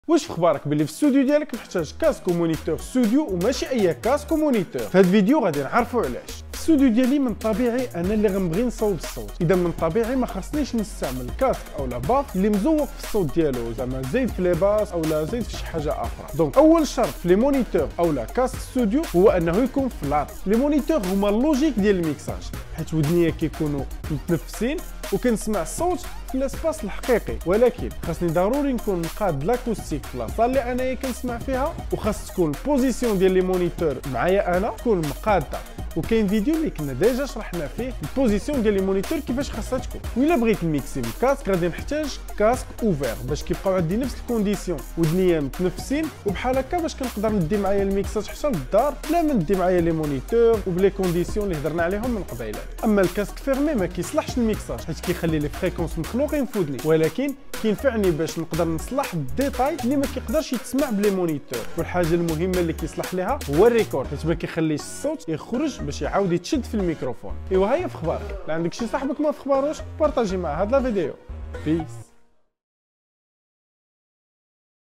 Wach Fkhbarek! Casques et Moniteurs Studio - Rock.ma
Voici l'importance des moniteurs et casques studio!

RockMa Musique! le plus grand magasin d'instruments de musique au Maroc